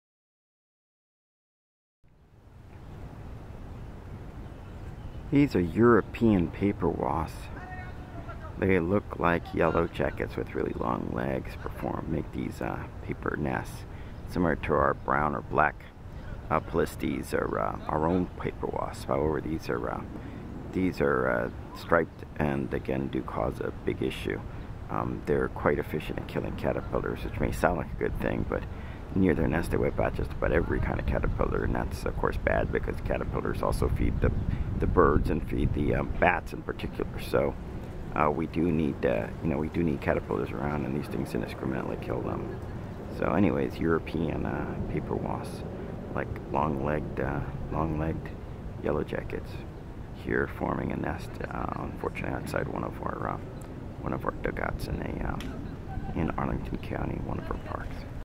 Capital Naturalist: European Paper Wasp Nest
European Paper Wasps caring for their nest. These are considered an invasive species, destroying numerous insects such as caterpillars and out-competing our native paper wasps.